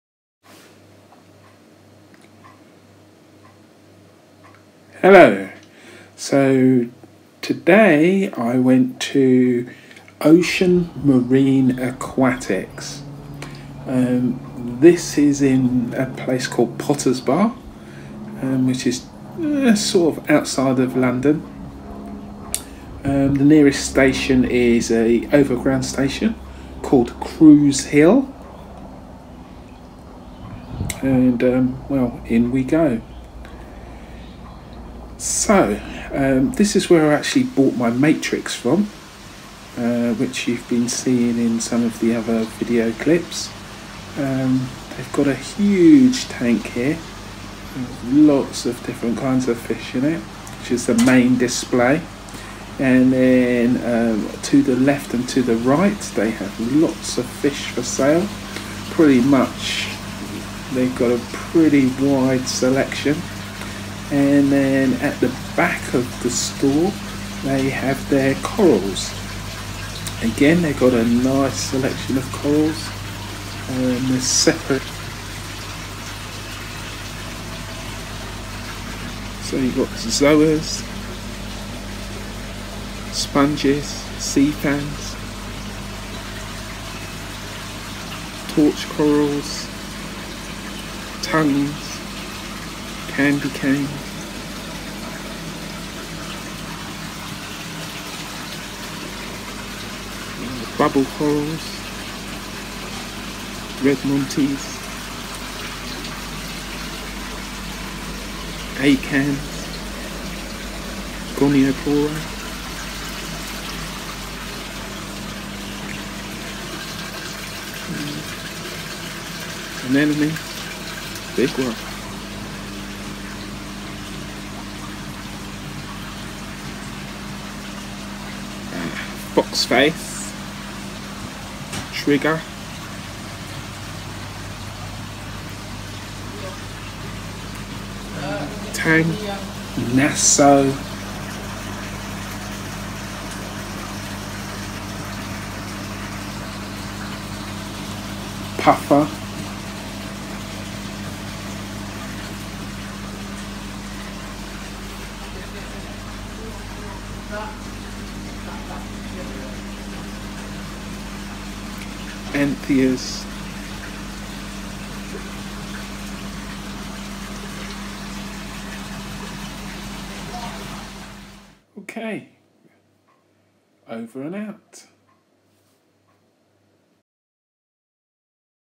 Ocean Marine Aquatics
The first time I found Ocean Marine Aquatics I was in Potter bar with a hours to spare. so I thought I would see if there were any  aquatic stores   around.
Who would have guessed in Enfield  I found  three stores within a mile of each other .
the first store i visited was Ocean Marine Aquatics.

Location:
Ocean Marine Aquatics Ltd.
Unit 14, Culver Garden Centre
Crews Hill
Enfield
EN2 0DS